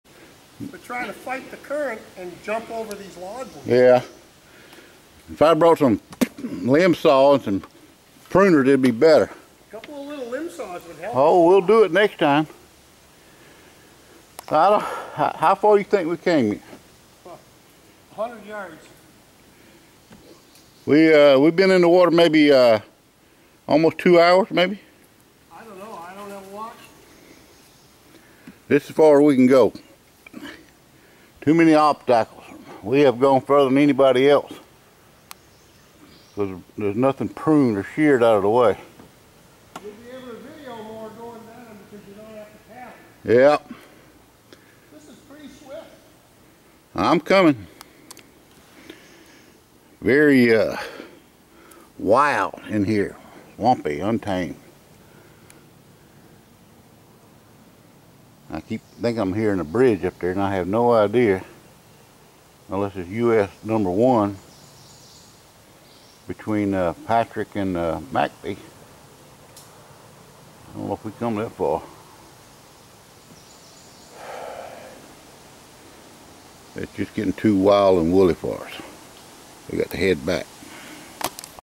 Black Creek Return Point
We had paddled up stream on Black Creek from lake Robinson. We came a good ways but it was starting to get wild and woolly in there and the we needed to either portage around some stuff or cut a way through which we were not prepared for. So, we opted to turn around. We were just down stream a ways from the bridge at Hwy #1. But man oh man what memories I have of this place as a boy.